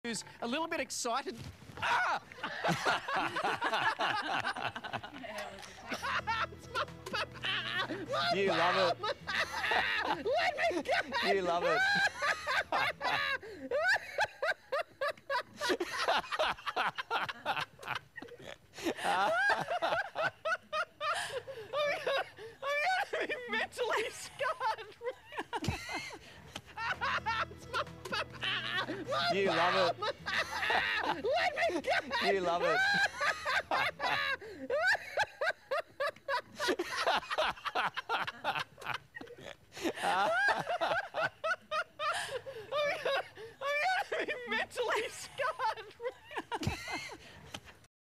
Pelican Attacks Reporter - Australia
February, 2010.

One weatherman in Australia may want to watch his back a little closer...After a pelican made him the laughing stock of a news show!

Steve Jacobs was  about to give a weather report earlier this month at the Taronga Zoo in Sydney when a bird got a  little frisky with him. Here you can see Jacobs pinned against the wall by the pelican... getting a pecking in his backside!

Fortunately, the bird's handler eventually got it to back off and Jacobs was unharmed in the avian assault. But, the weather man joked he will now be quote,  "emotionally scarred" by the attack.